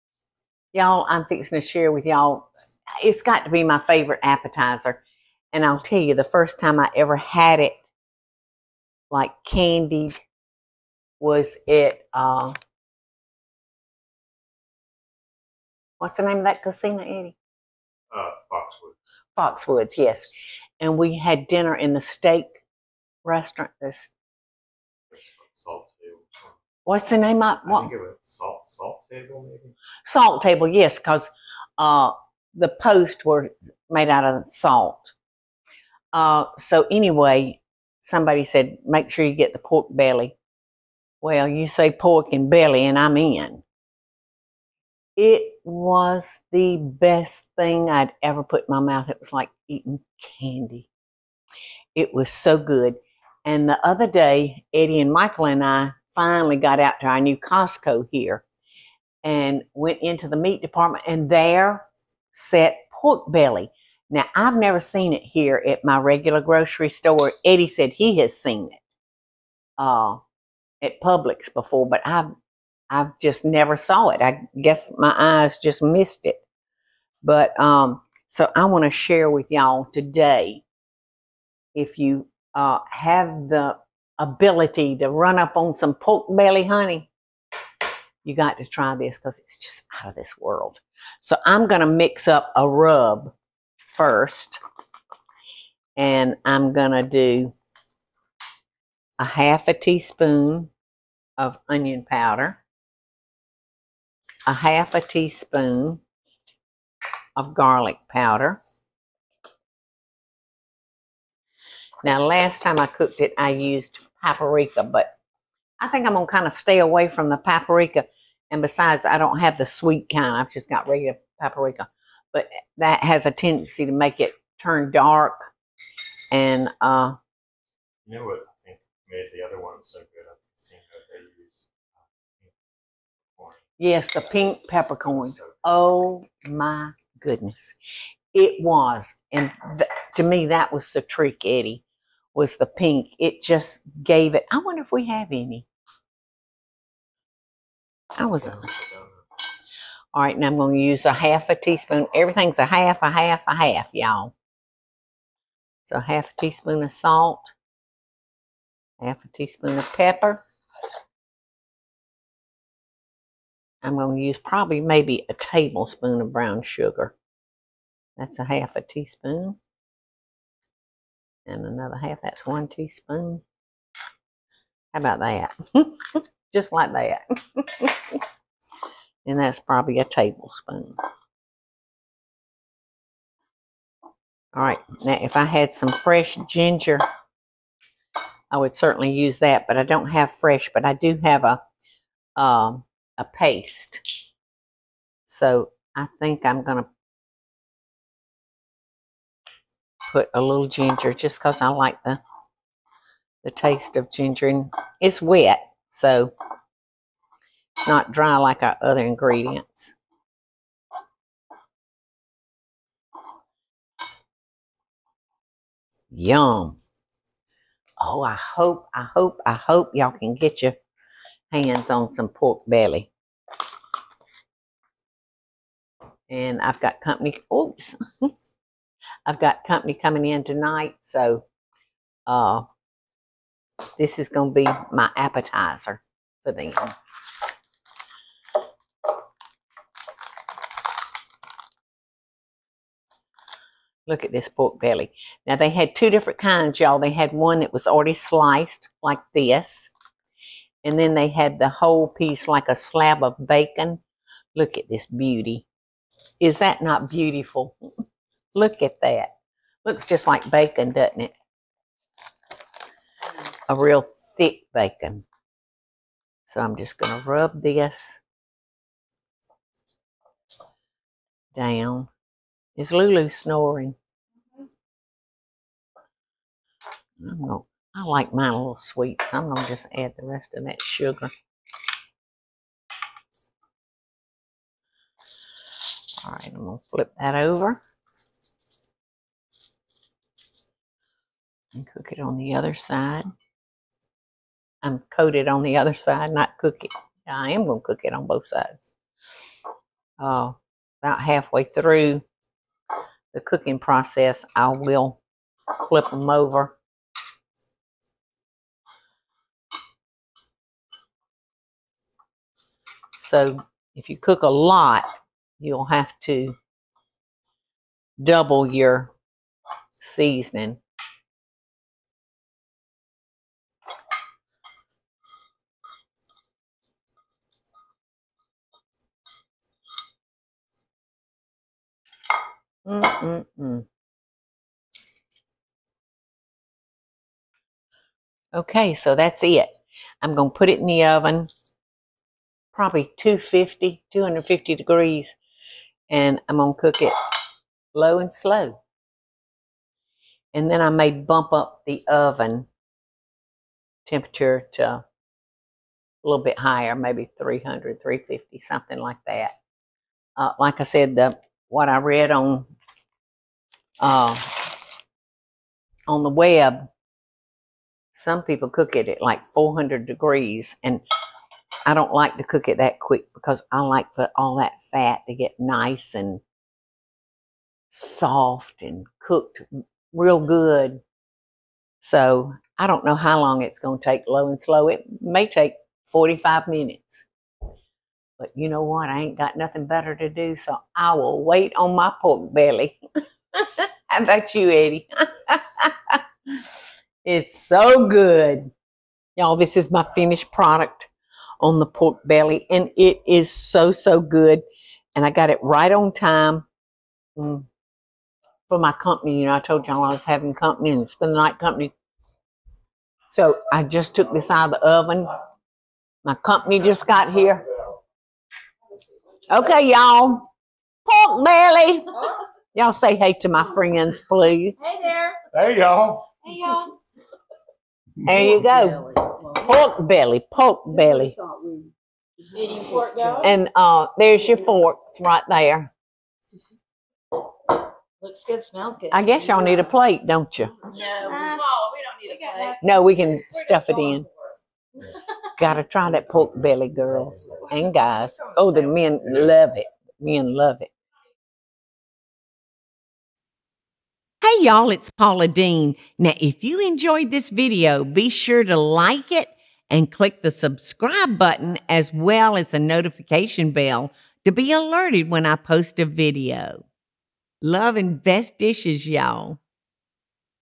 Love & Best Dishes: Baked Pork Belly Recipe | Candied Pork Belly Appetizer
Baked Pork Belly Recipe | Candied Pork Belly Appetizer - Paula's sharing how to cook pork belly with this deliciously sweet caramelized pork belly recipe.
Sign up for my email mailing list and get 3 FREE helpful kitchen printables